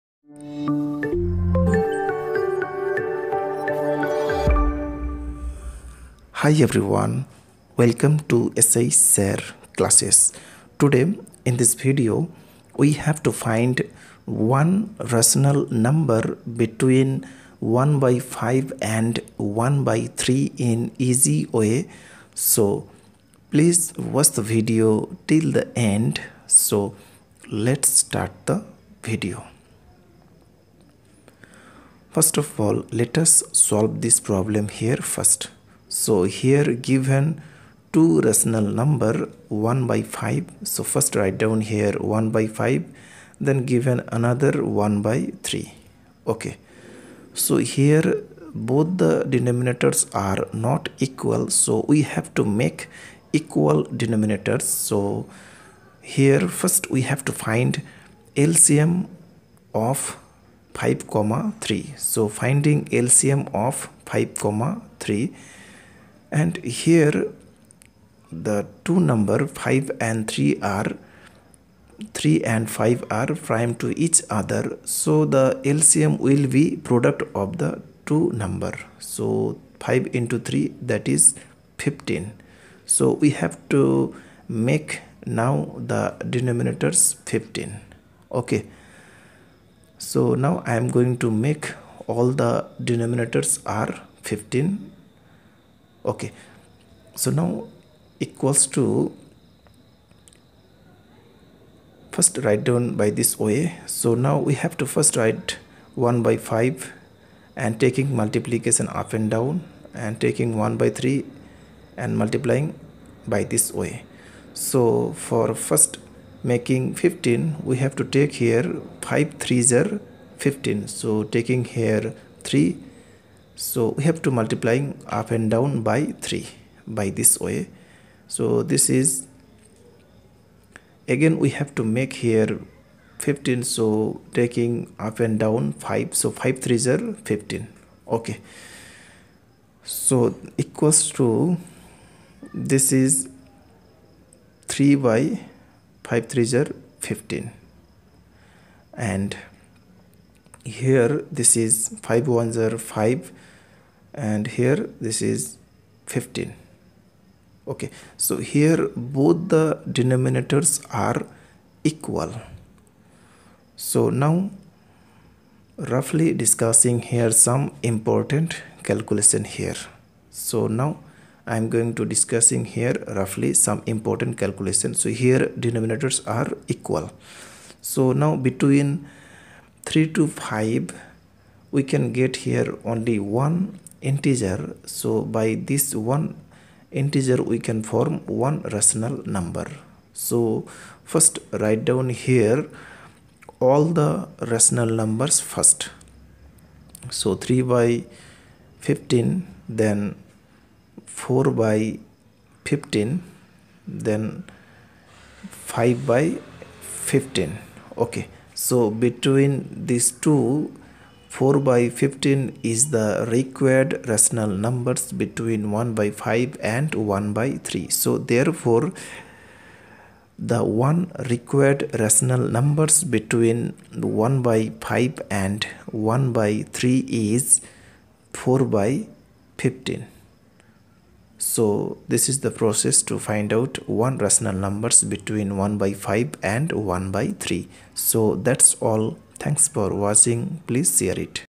Find one rational number between 1/5 and 1/3.@SHSIRCLASSES.
In Easy Way how to find one rational number between 1/5 and 1/3.@SHSIRCLASSES. Real Numbers.